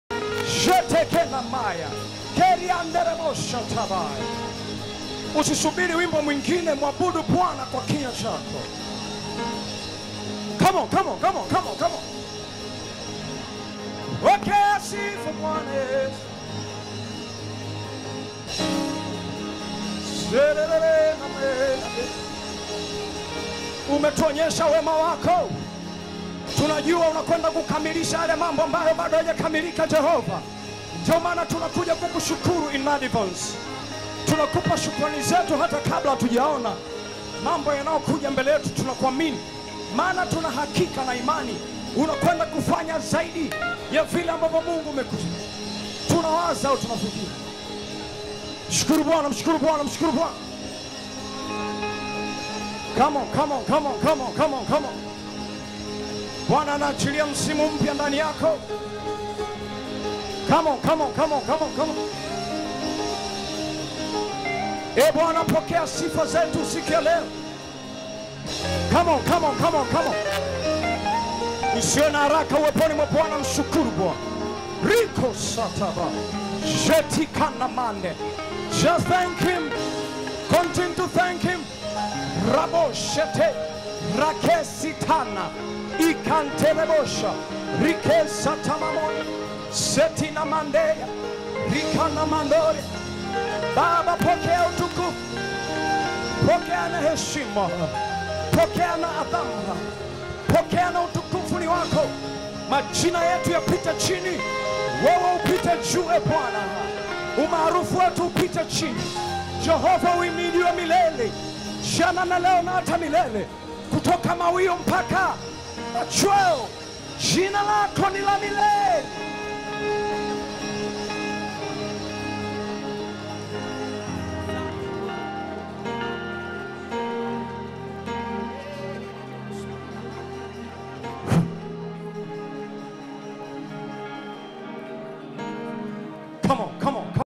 Dkt Ipyana Kibna anena kwa lugha kwenye Kongamano la Shilo
Dkt Ipyana Kibona anena kwa lugha kwenye Kongamano la Shilo siku ya Jumapili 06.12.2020 katika kanisa la Mlima wa Moto Mikocheni "B"